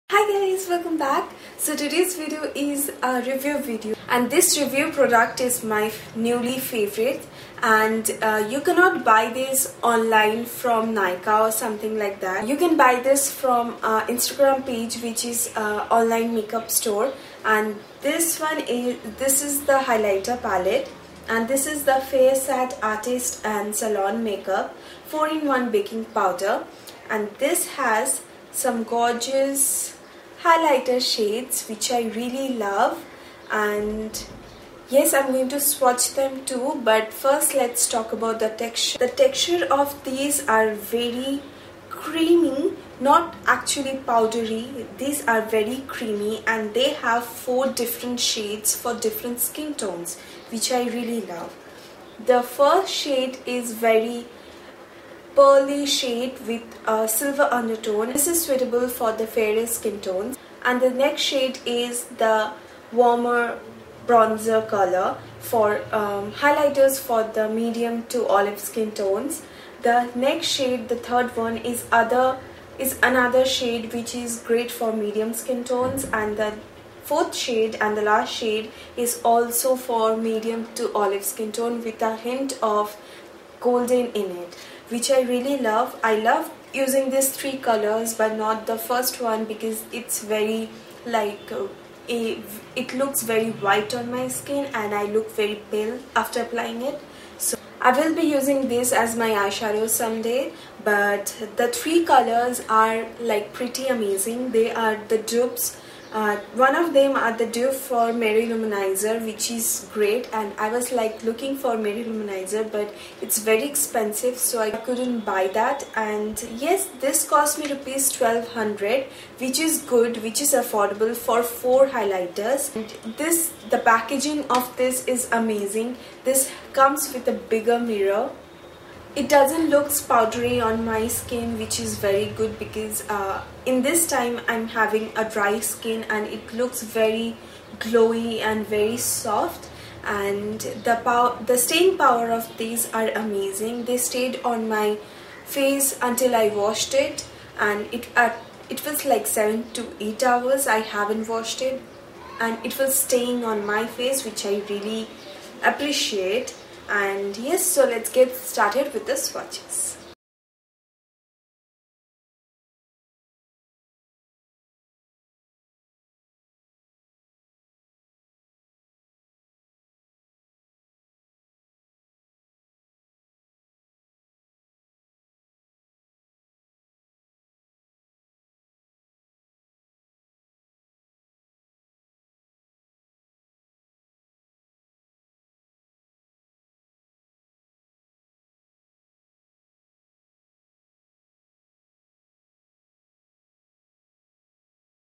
REVIEW- FARESAT highlighter palette
FARESAT HIGHLIGHTER PALETTE IS AMAZING AT RS 1200 YOU GET 4 SADES WHICH IS AMAZING.

BUY- online_makeup_store_india

TAKECARE.